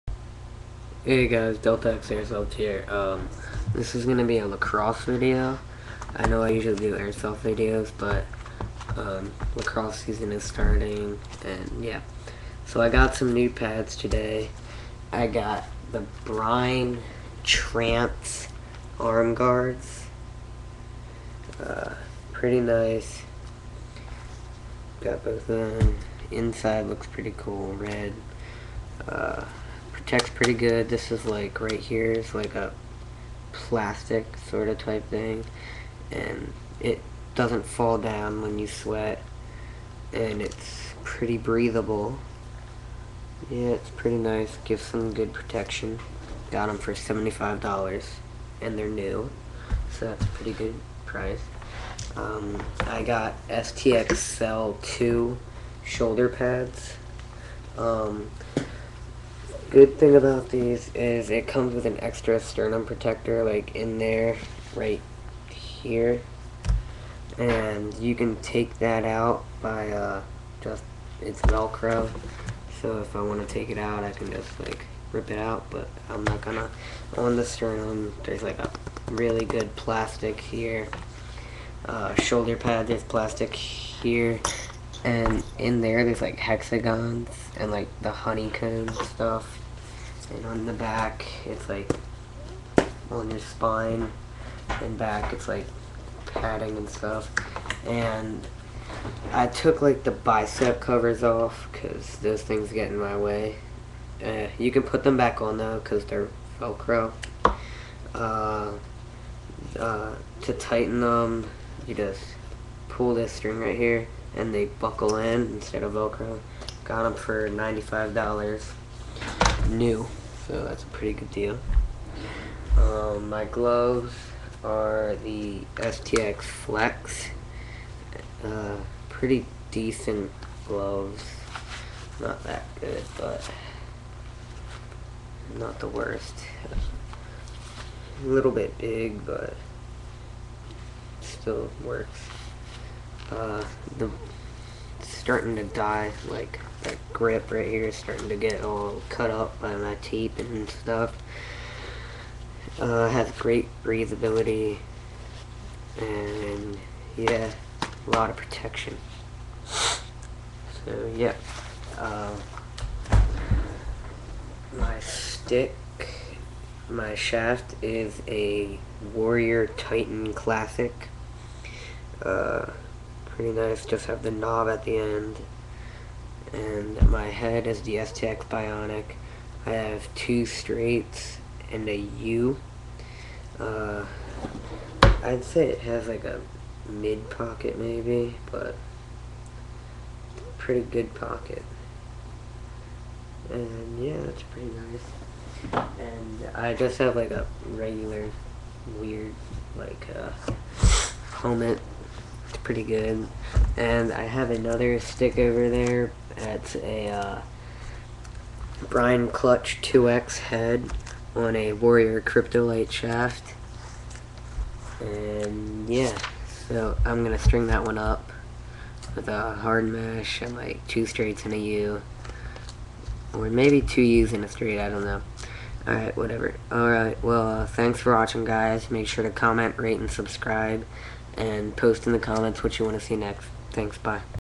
my lacrosse gear
Brine Trance Lacrosse arm guards: $70
Stx Cell 2 shoulder pads: $95
Stx gloves : $ 90
Brine clutch 2x: $100